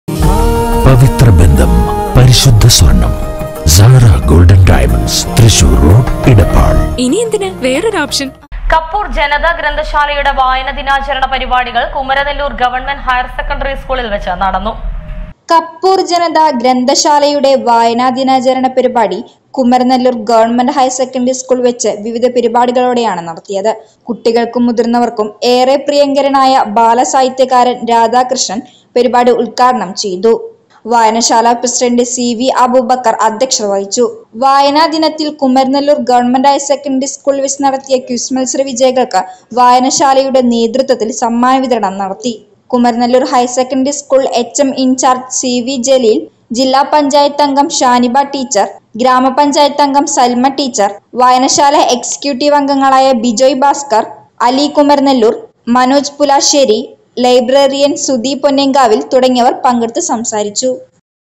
കപ്പൂർ ജനത ഗ്രന്ഥശാലയുടെ വായന ദിനാചരണ പരിപാടികൾ
കപ്പൂർ ജനത ഗ്രന്ഥശാലയുടെ വായന ദിനാചരണ പരിപാടികൾ കുമരനെല്ലൂർ ഗവ.ഹയർസെക്കണ്ടറി സ്‌കൂളിൽ വെച്ച് നടന്നു